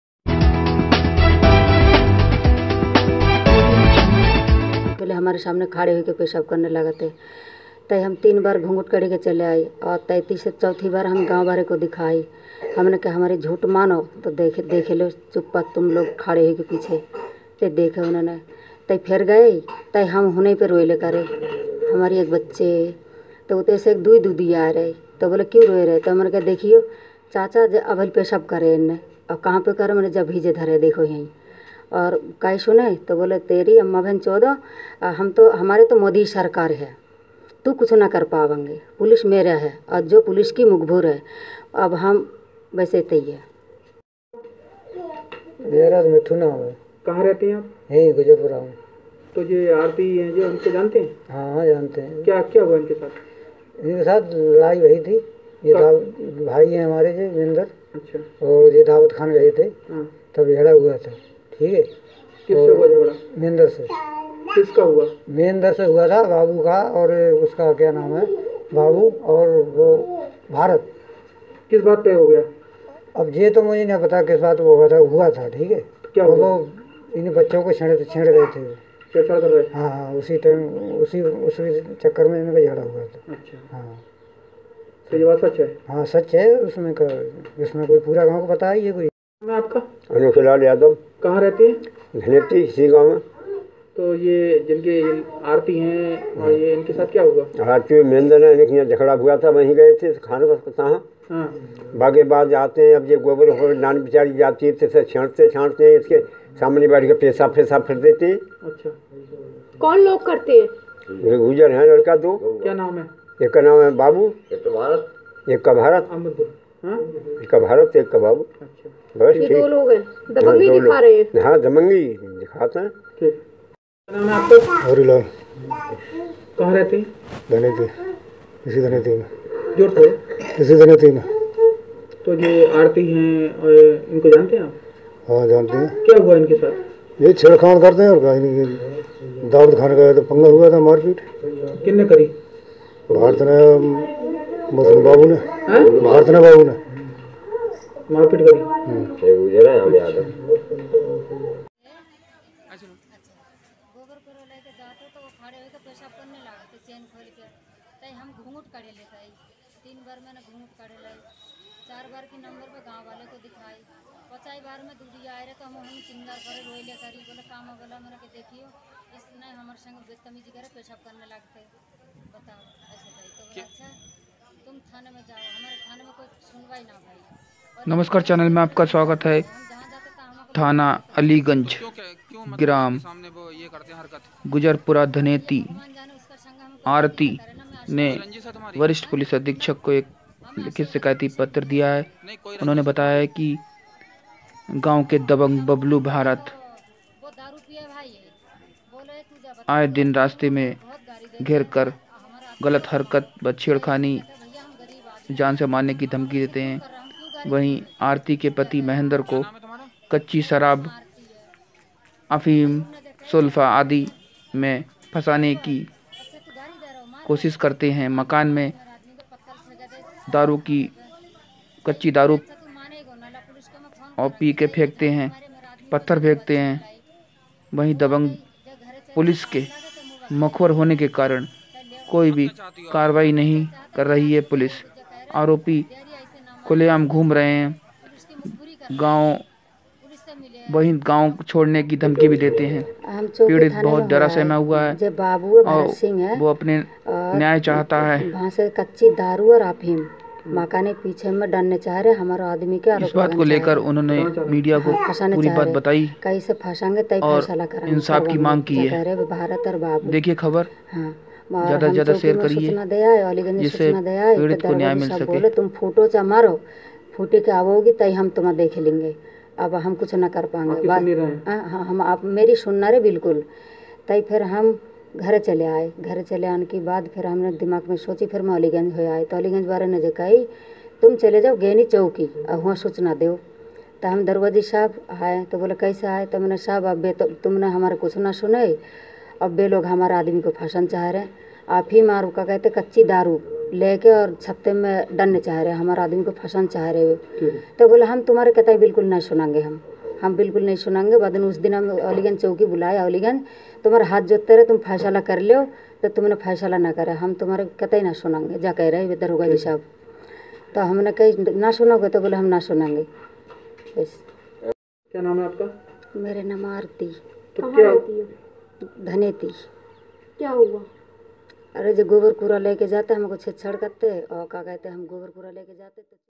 RGAन्यूज़:थाना अलीगंज गुर्जर पुरा धनेती गांव के दबंग आए दिन करते हैं दबंगई
बरेली थाना अलीगंज गुर्जर पुरा धनेती गांव के दबंग आए दिन छेड़खानी रास्ते में रोकता है वही आरती के पति को दारू कच्ची सुल्फा अफीम में फसाने की कोशिश करता है गैनी चौकी अलीगंज थाना पीड़ित की कोई सुनवाई नहीं कर रहे हैं पीड़ित आरती ने वरिष्ठ पुलिस अधीक्षक  को लिखित शिकायती पत्र दिया है इंसाफ की मांग की है